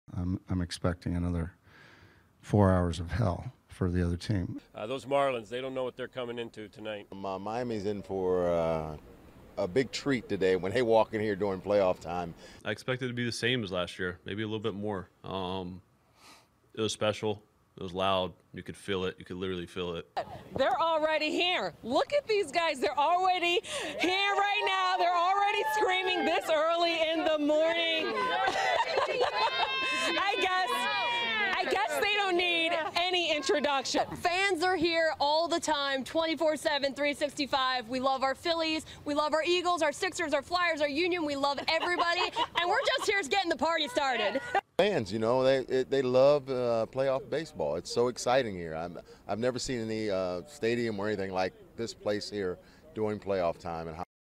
"Another 4 hours of hell": Red October takes over Citizens Bank Park for Phillies-Marlins Game 1
From Rob Thomson to Zack Wheeler and 1993 NL champions Mickey Morandini and Milt Thomson, the Phillies are expecting a raucous crowd at Citizens Bank Park on Tuesday night.